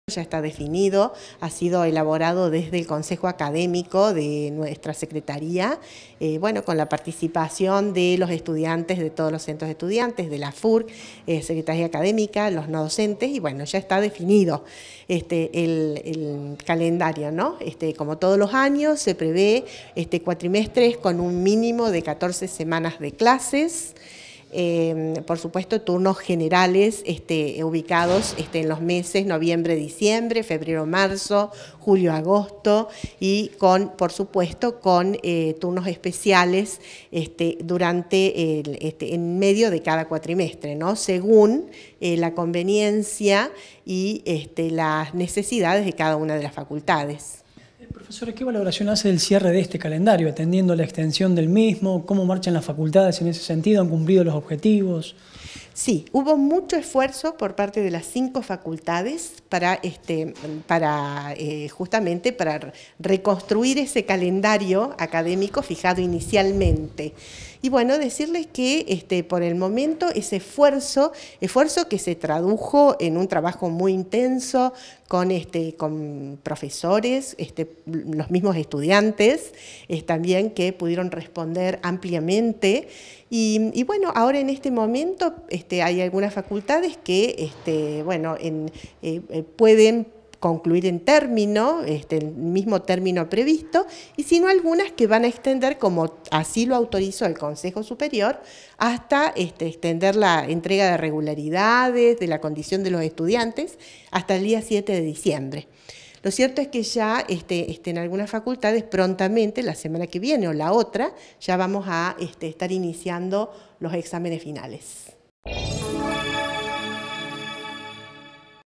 CALENDARIO 2019 - ANA VOGLIOTTI
FIJARON FECHAS DE INICIO Y CIERRE DE CUATRIMESTRES Y TURNOS DE EXÁMENES
Facebook.com/uniriotv
Twitter.com/uniriotv